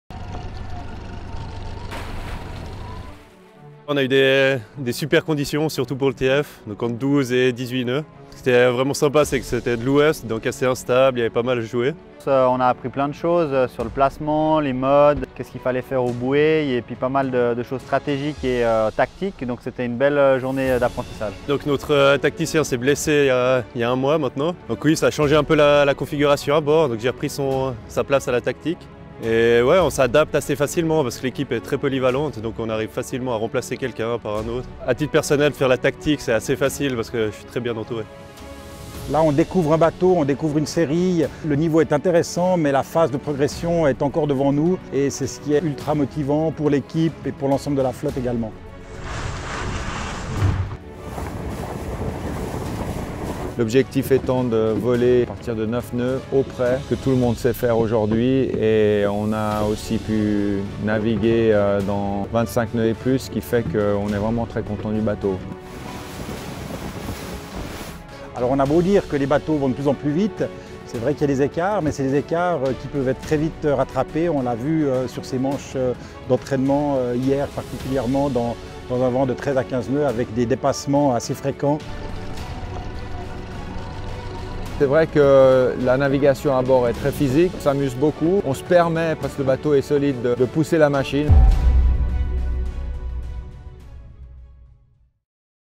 2020 - TF35 test event 2 - Société Nautique de Genève
L'équipe évoque le 2e test event des catamarans foilers TF35 tenu du 9 au 11 octobre sur le lac Léman.